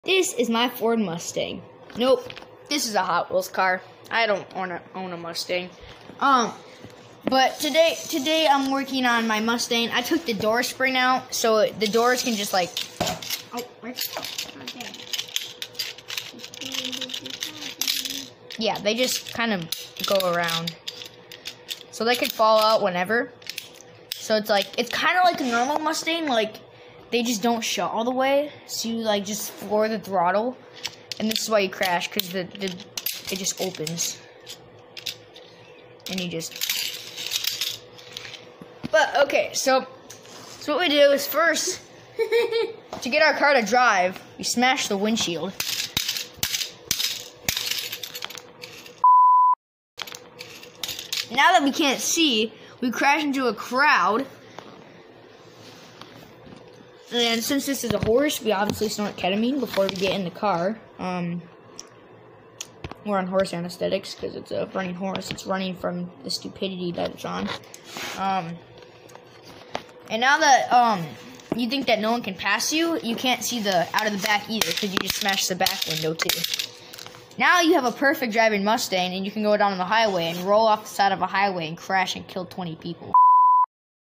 How To Drive A Mustang, Correctly...
You press the left pedal, then pull the shaft in the middle of the vehicle, then press the right pedal as hard as you can. Let off the left pedal, and never use the middle pedal.